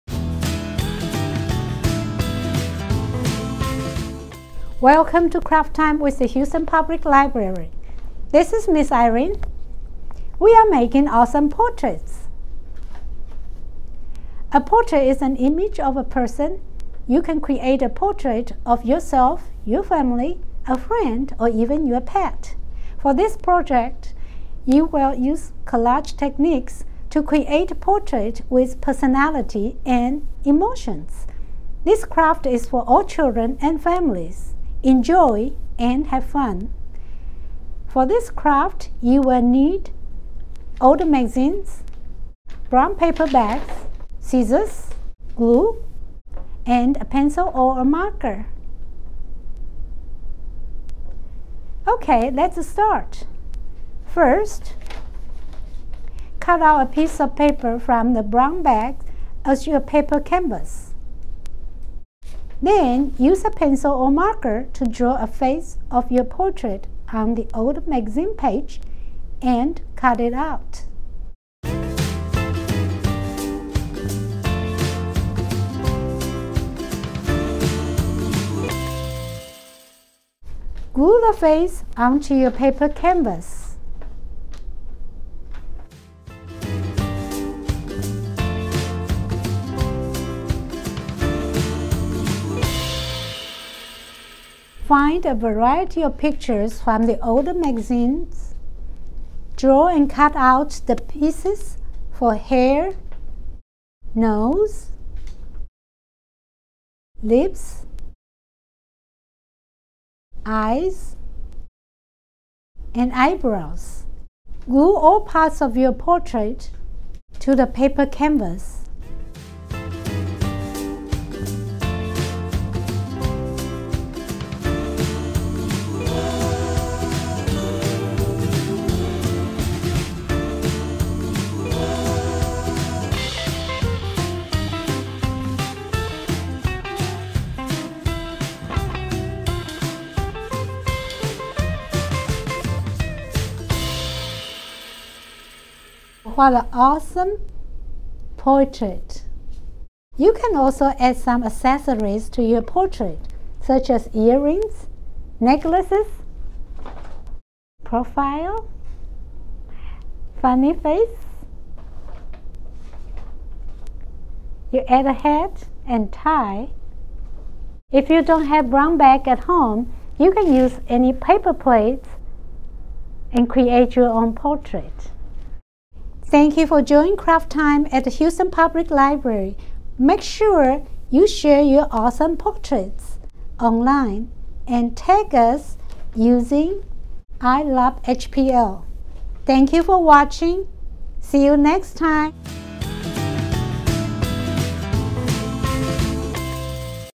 HPL Craft - Awesome Portraits
Do you have a brown paper bag and some old magazines at home? Yes? Alright then! Get ready for an amazing craft shared by Ms. Irene with the Houston Public Library.